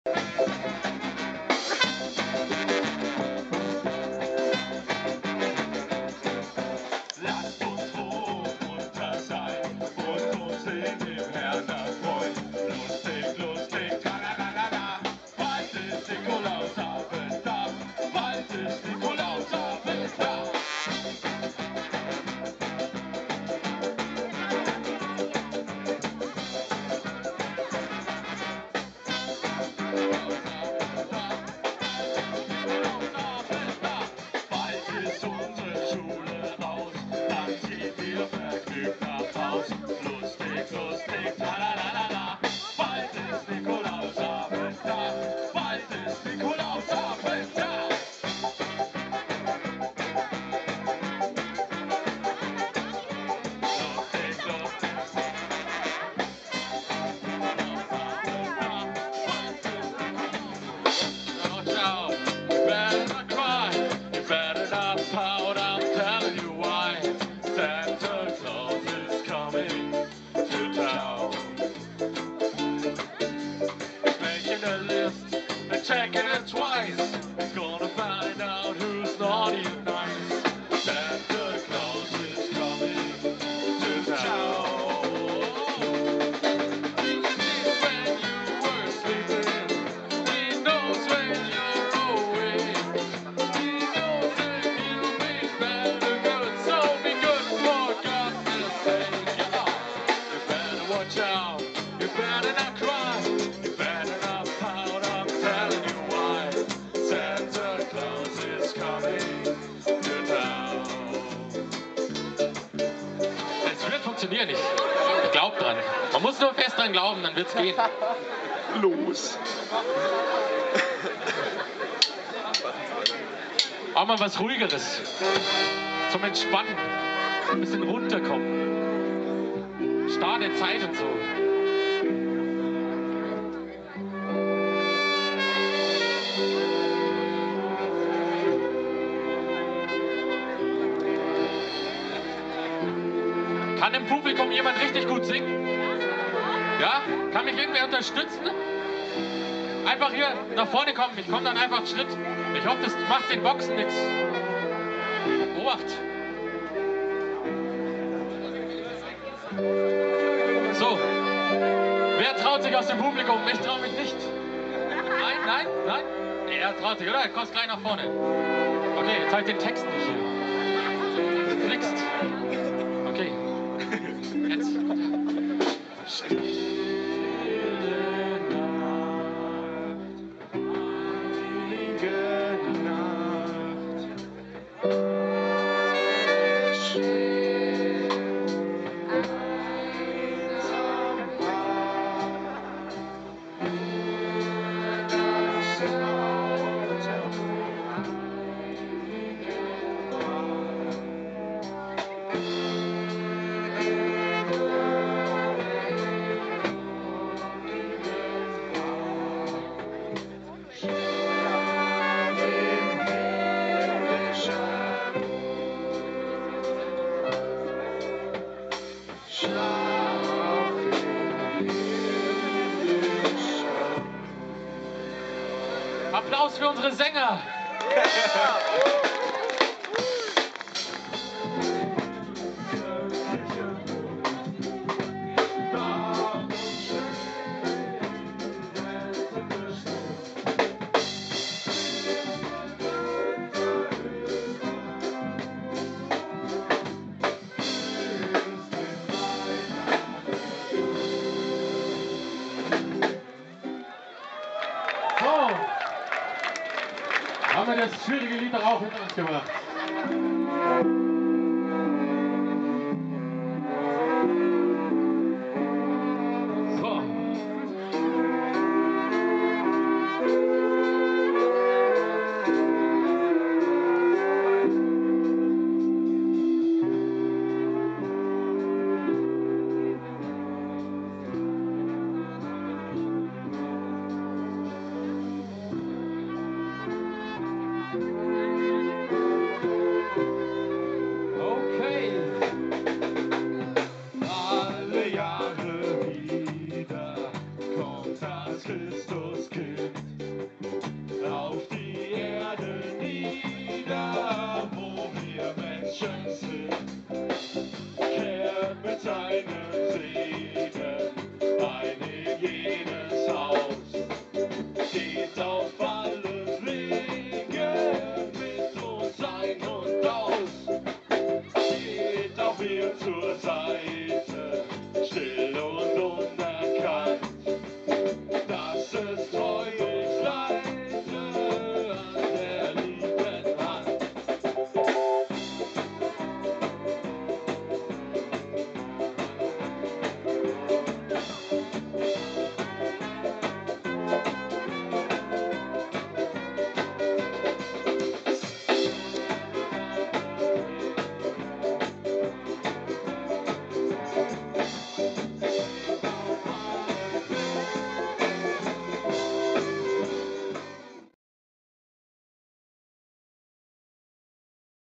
Weihnachtskonzert SKABONES
Skabones Weihnachtskonzert auf dem Christkindlmarkt Pfaffenhofen
Skabones Weihnachtskonzert auf dem Christkindlmarkt Pfaffenhofen. Wenn der Weihnachtsreggae kommt. Das Konzert schlechthin auf der Bühne des Weihnachtsmarktes 2010.
Die ultimative Antwort auf den Festtagsstress, der Christkindlmarkt tanzt. Gut 1000 Besucher aller Altersklassen beim Konzert der Pfaffenhofener Band SKABONES feierten bei bester Laune.